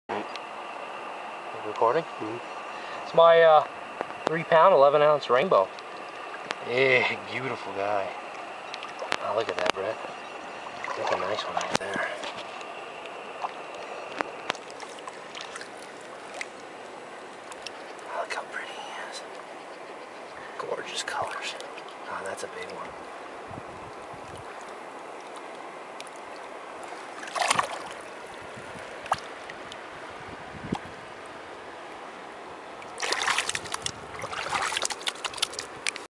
Big 4lbs Rainbow on the Truckee River
a nice 4lbs rainbow caught east of Reno in spring of 2012. this is an exceptionally beautiful rainbow caught on the Truckee river, long, fat and extremely spotted. its another one of our nice trout catches in the sierra nevada